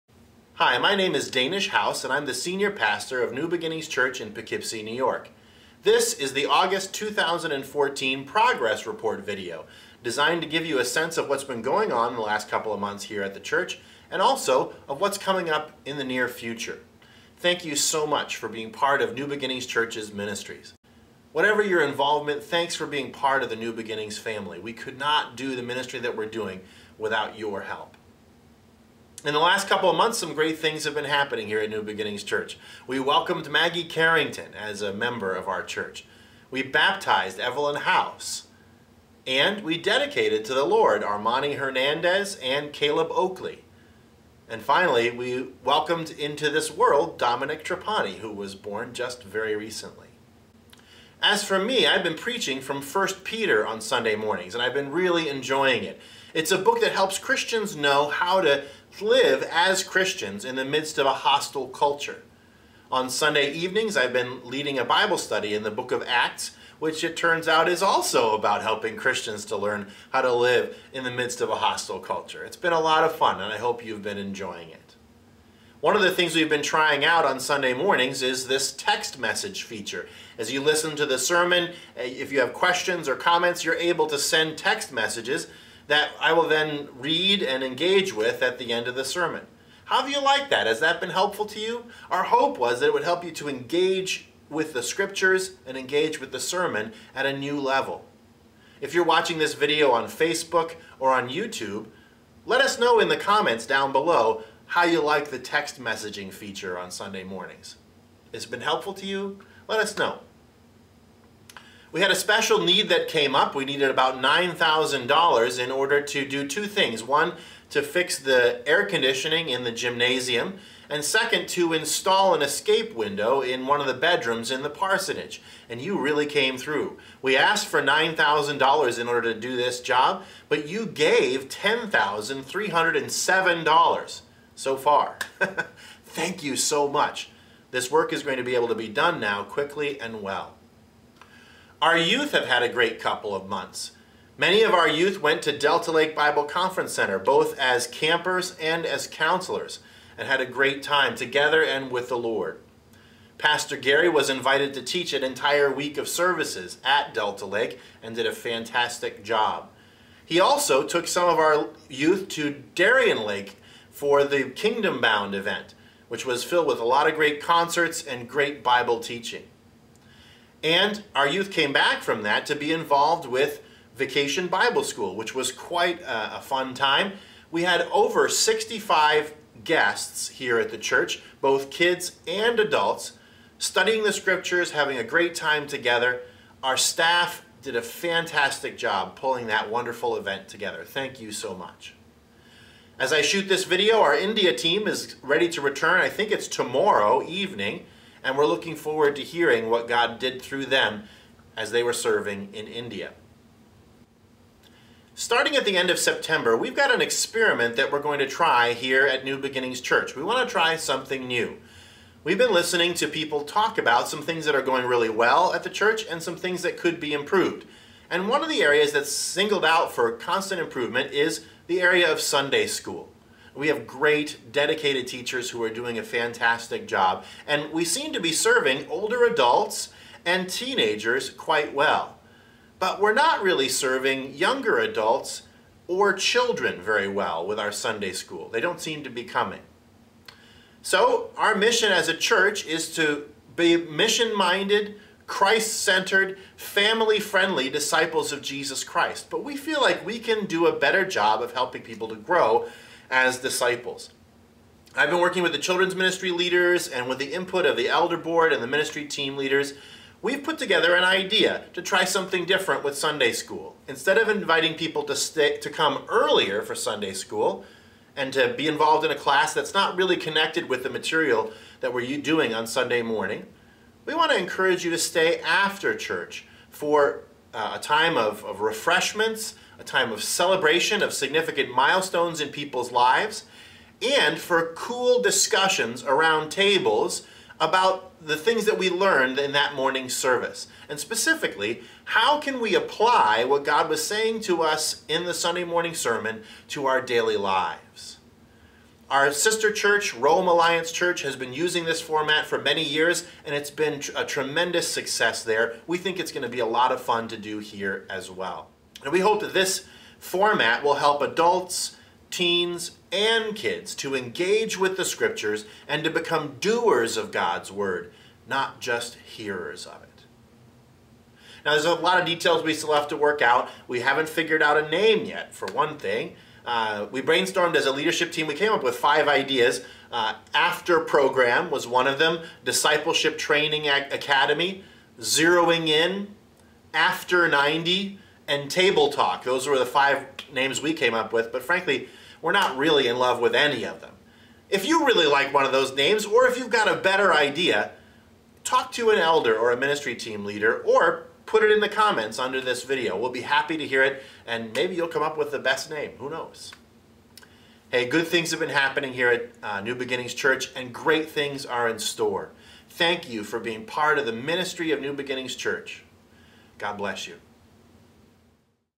2014 August Progress Report
This is our August 2014 Progress Report! Including a look at what will be happening with Sunday School in the coming quarter.